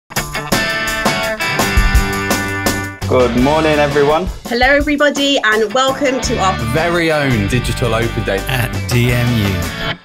What to expect from a digital DMU Open Day
Join us for a De Montfort University Leicester (DMU) Digital Open Day on Saturday 19 September or Saturday 17 October and hear from academics and current students about studying with us in Leicester.